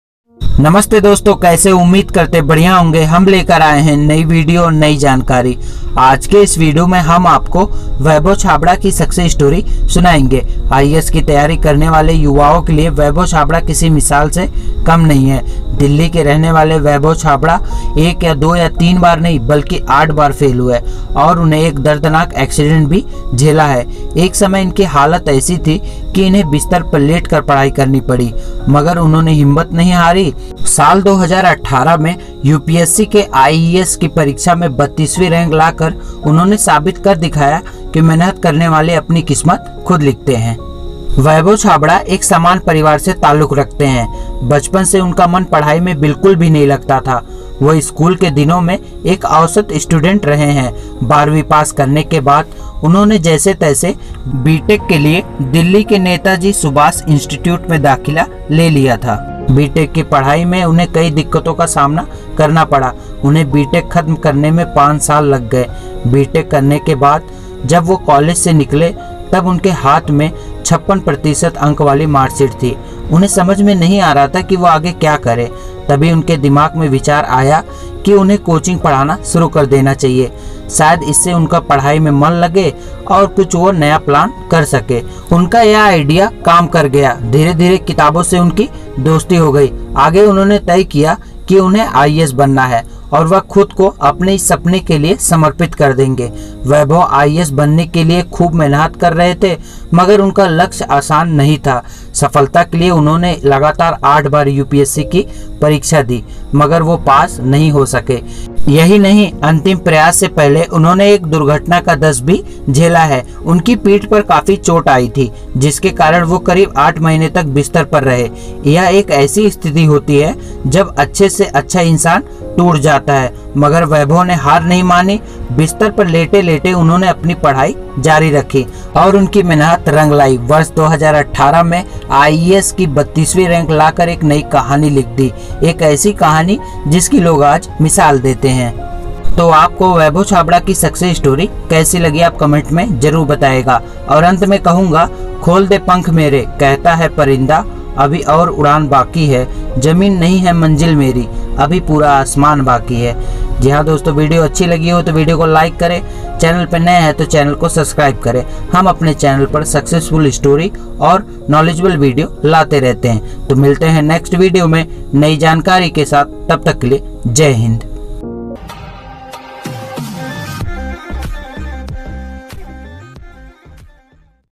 IES Vijay Chhabra success story 🤨🤨8 बार फेल हुए दर्दनाक एक्सीडेंट झेला बिस्तर पर लेटकर की पढ़ाई
IES  Vaibhav Chhabra success story 🤨🤨8 बार फेल हुए दर्दनाक एक्सीडेंट झेला बिस्तर पर लेटकर की पढ़ाई

          दोस्तों आज की इस वीडियो  वैभव छाबड़ा की सबसे स्टोरी सुनाइए कैसे वह यूपीएससी के एग्जाम में एक दो बार नहीं बल्कि 8 बार फेल हुए तब पर भी उन्होंने हार नहीं मानी और अंत में वैभव ने यूपीएससी एग्जाम पास करकर IES ऑफिसर बने

my mobile
my boya mic